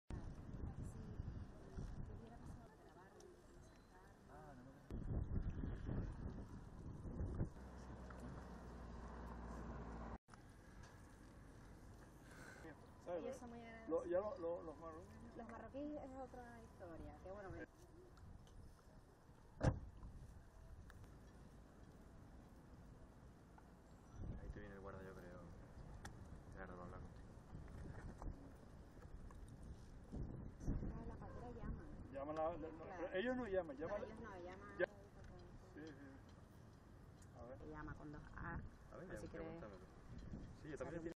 Dos personas desaparecidas y más de 160 rescatadas en las últimas horas
Dos personas han desaparecido durante la madrugada de este lunes en aguas próximas a Canarias, donde efectivos de Salvamento Marítimo y de dos buques han rescatado a más de 160 migrantes en las últimas horas.

Informa: Redacción Televisión Canaria